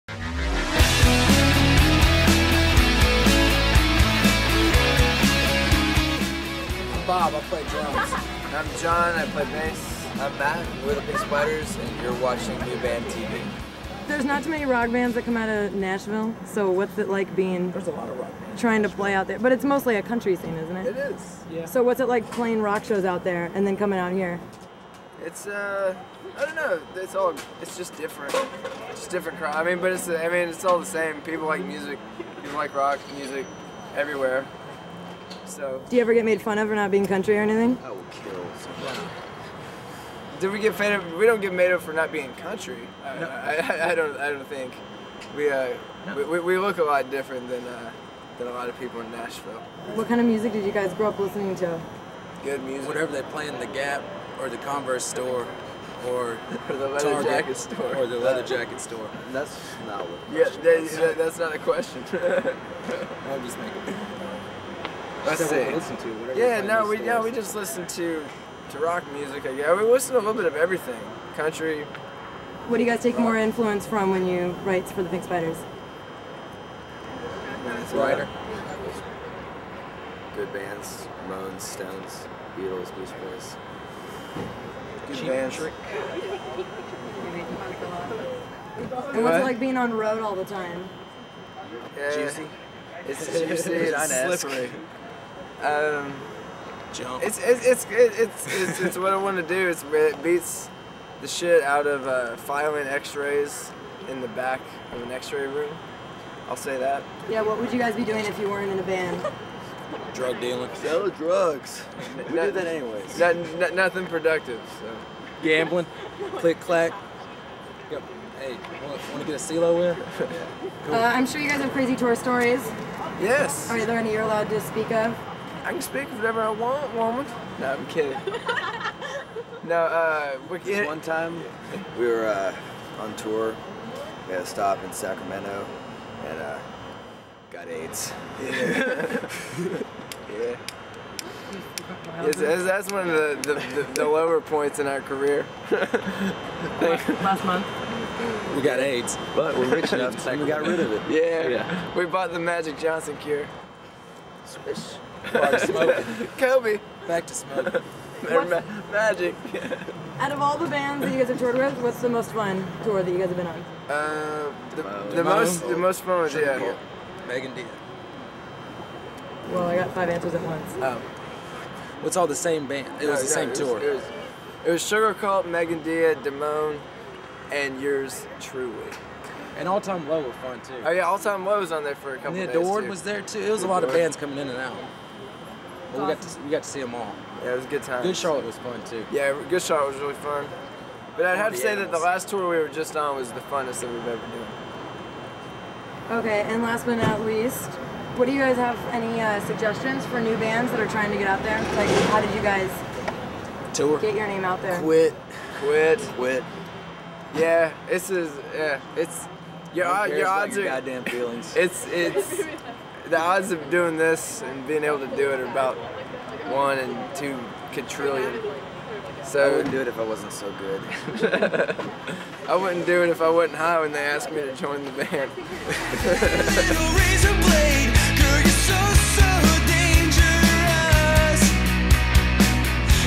The Pink Spiders Interview with NewBandTV.com
The guys talk about good music, filing x-rays and the Magic Johnson cure.